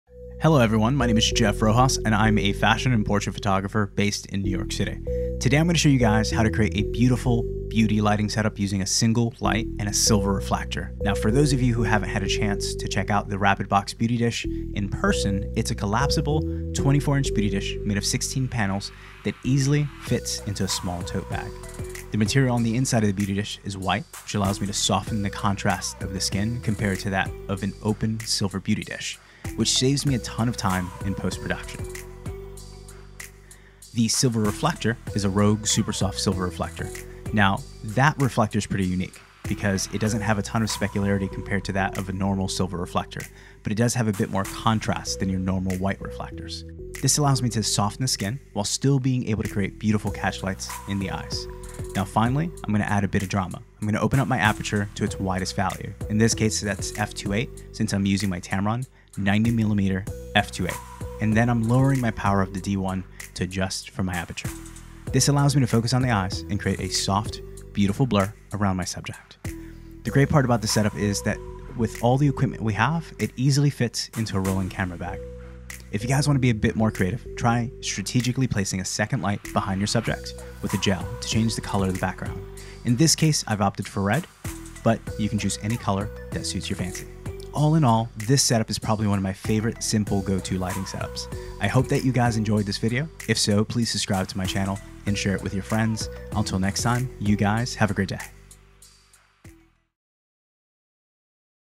Studio Lighting – One Light Beauty Setup
Here's a simple way to create a beauty image, using a single light. It's also an exclusive look at the new Westcott Switch Beauty Dish! :D
–THE EQUIPMENT I USED–
Light Modifier